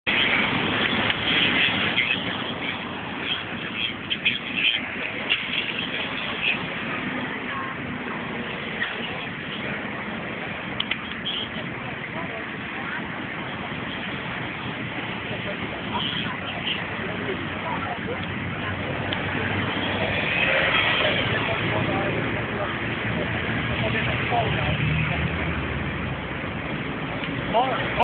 Powódź Tarnów 04.06.2010 - most na rzece Biała , godzina 17:30
Powódź Tarnów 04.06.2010 - most na rzece Biała , godzina 17:30
jako że policja zablokowała dojście na most filmik został nakręcony kilka metrów przed mostem